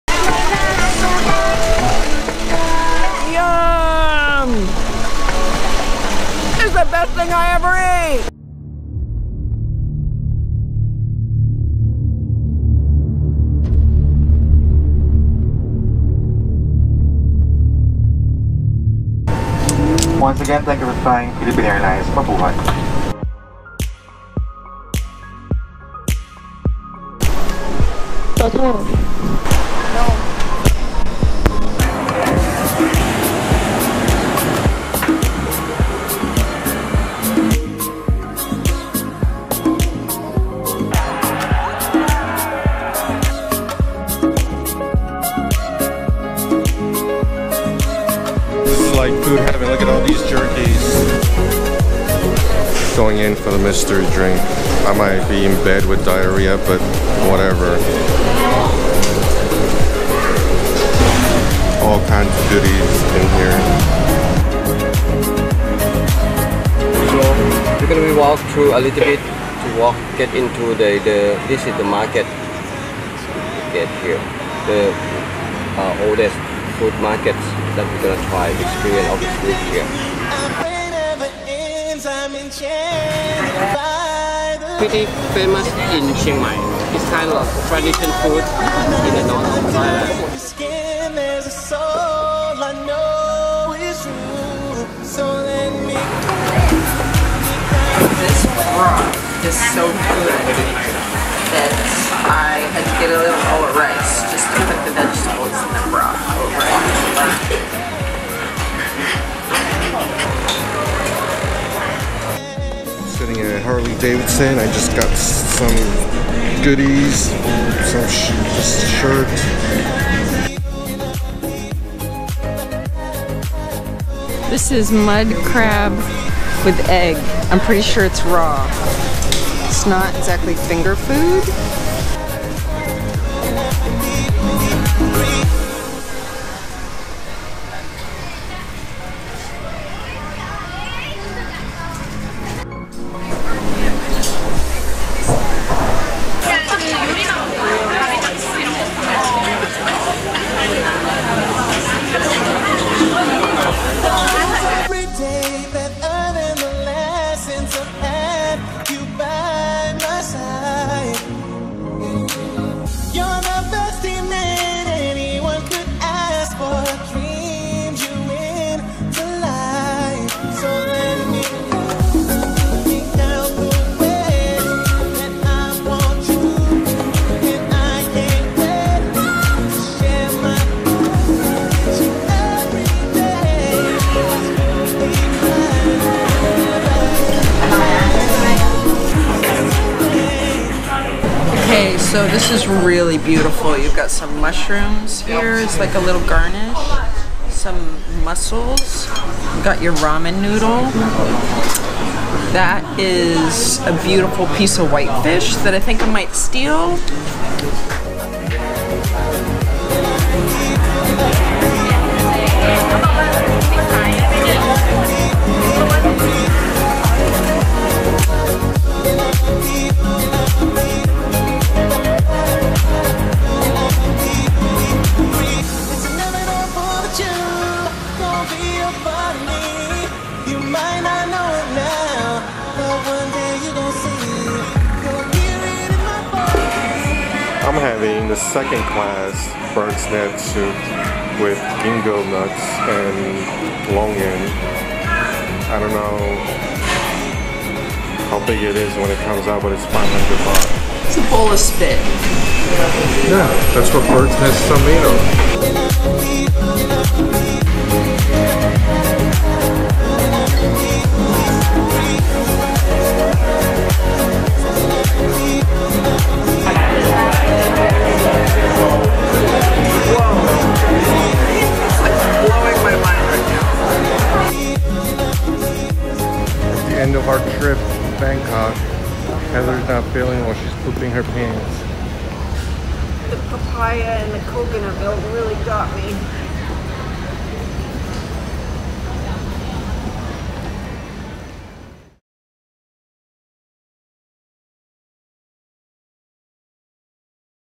Highlights of our trip to Bangkok in Sept 2018
Awesome trip to Bangkok, Thailand. Visited Rod Fai night market, visited a few places recommended by Mark Wiens from boat noodles (Doy Kuay Teow Reua ต้อยก๋วยเตี๋ยวเรือ), braised beef (Neua Toon Tha Nam Sathu Pradit ร้านเนื้อตุ๋นท่าน้ำสาธุประดิษฐ์).

Had dinner at Jim Thompson house, watched a kickboxing fight at Rajadamnern Stadium!

Loved Bangkok, can't wait to go back!!